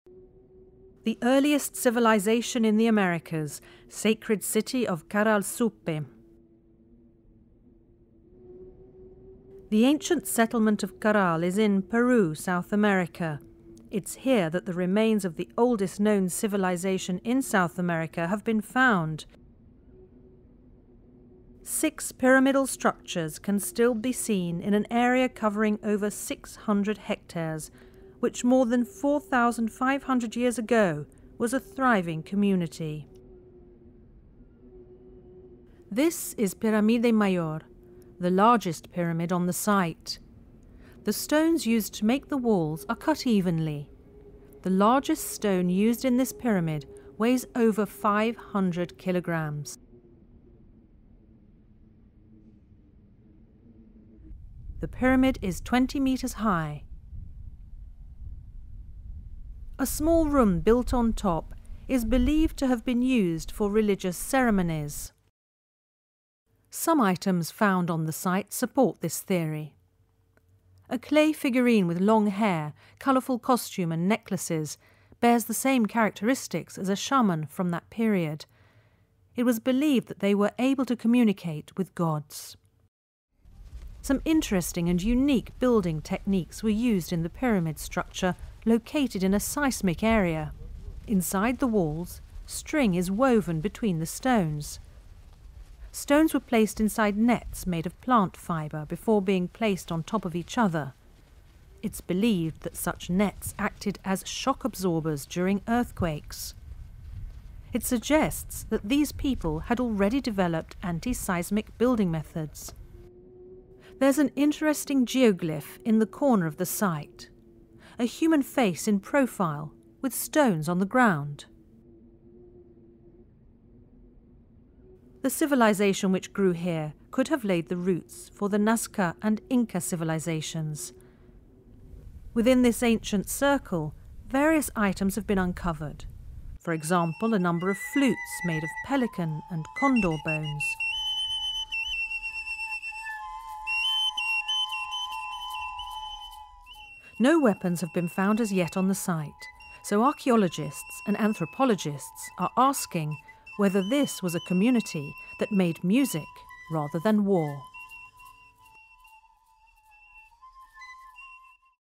Sacred City of Caral-Supe (UNESCO/NHK)
The 5000-year-old 626-hectare archaeological site of The Sacred City of Caral-Supe is situated on a dry desert terrace overlooking the green valley of the Supe river. It dates back to the Late Archaic Period of the Central Andes and is the oldest centre of civilization in the Americas. Exceptionally well-preserved, the site is impressive in terms of its design and the complexity of its architectural, especially its monumental stone and  ...